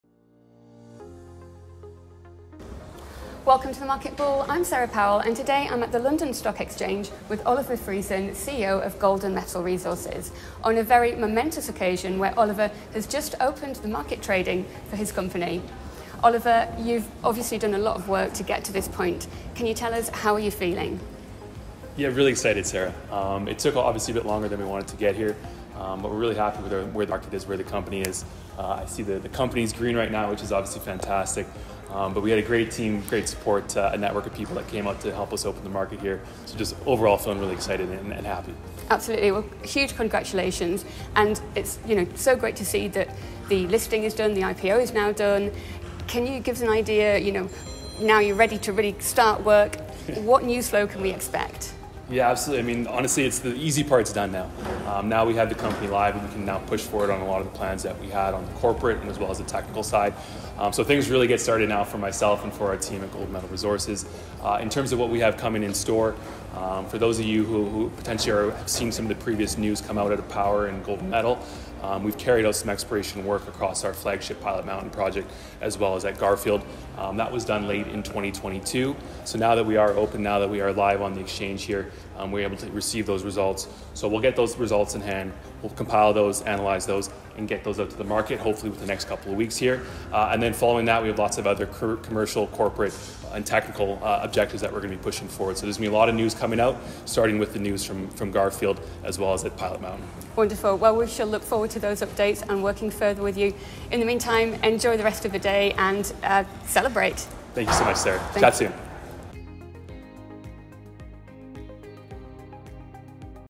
Exclusive Interview: Golden Metal Resources CEO on Successful Listing on London Stock Exchange
SarahPowell is joined by Golden Metal Resources CEO, Oliver Friesen to capture his excitement following the Company's successful listing on the London Stock Exchange! 📊👏🏼